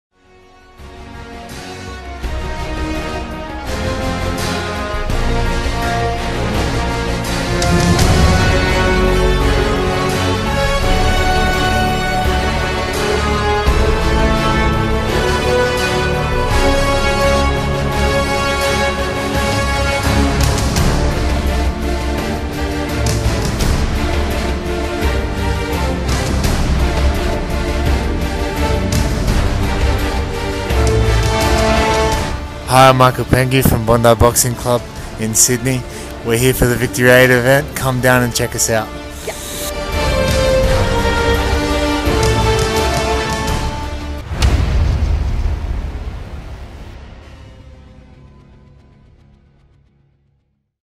Michael Pengue @Victory8 Legends of Hoan Kiem
Mickey is class boxer that came to Vietnam in November 2018 and showed his skills with a dominant win over the courageous Vo Hong Dat. He has had five wins as a professional – four of those ending in knockout. His most recent performance was undoubtedly his best – a dominant display ending in stoppage against the very strong Isaac Buckley. The win earned Mickey Pengue the Australasian title, and he ventures to Vietnam in October in top form.